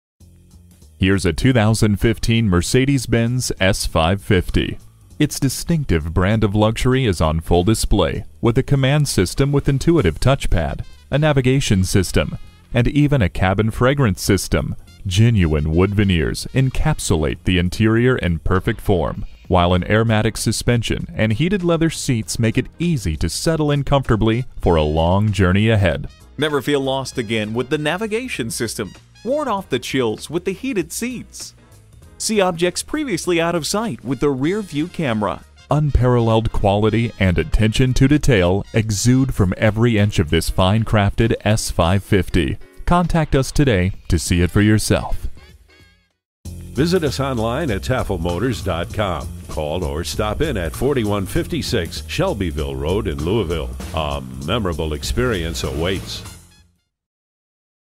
2015 Mercedes-Benz S-Class Louisville KY Elizabethtown, KY US9547 - SOLD
Year: 2015
Make: Mercedes-Benz
Model: S-Class
Trim: S550 Sedan
Engine: 8 Cyl. 4.7 L
Transmission: Automatic
Color: Black
Interior: Black
Mileage: 3534
Stock #: US9547
VIN: WDDUG8CB0FA165662
P

Address:
4156 Shelbyville Road